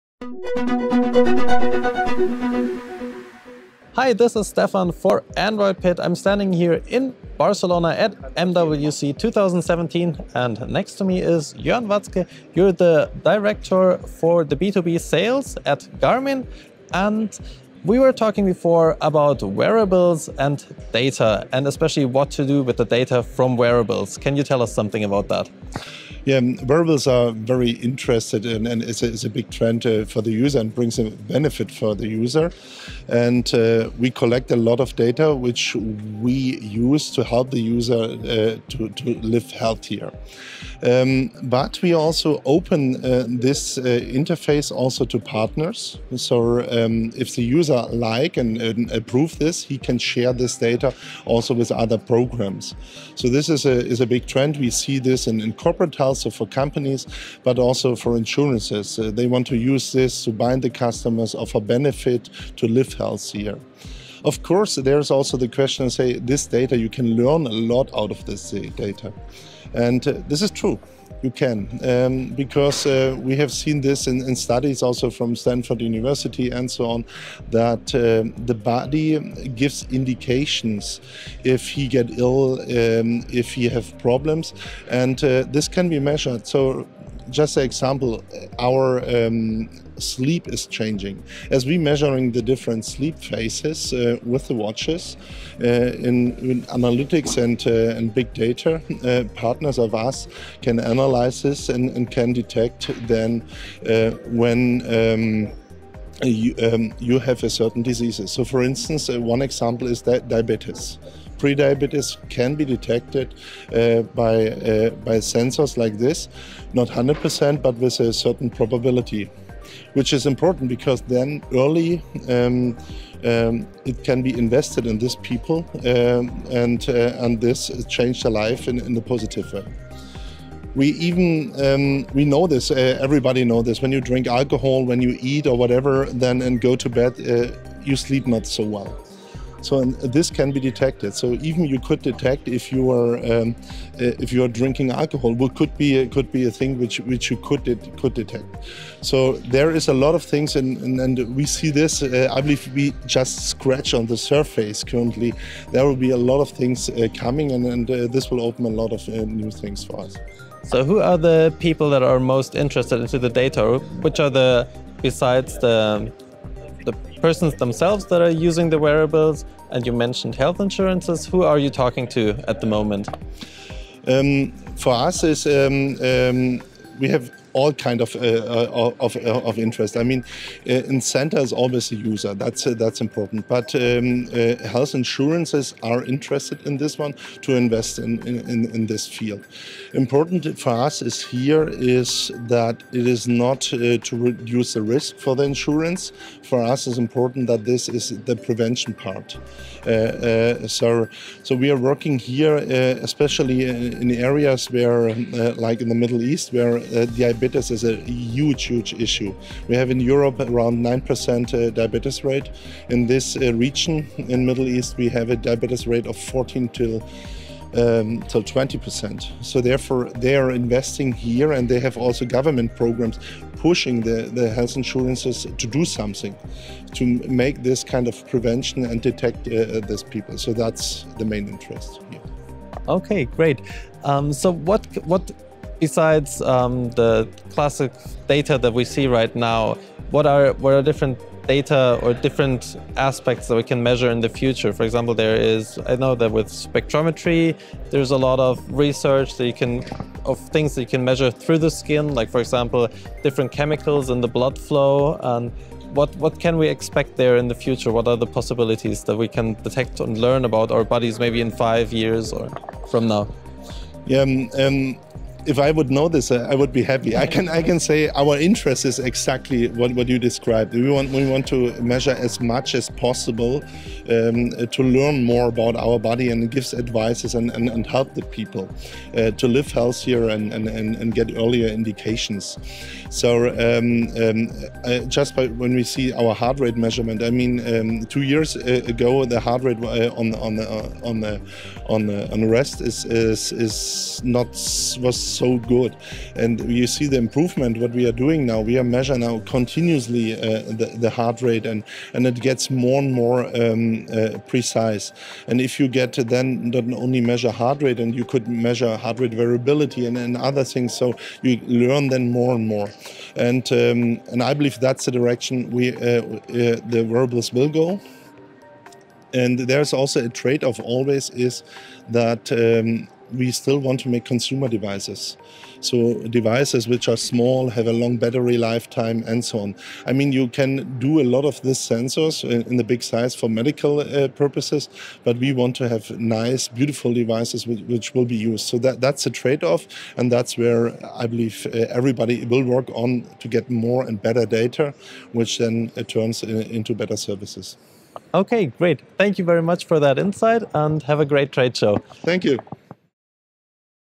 How your wearable knows you're getting sick – Interview with Garmin
About us:
Your source for Android smartphone news, reviews, comparisons, apps, forum, videos, tips + tricks for getting the most out of your Android device.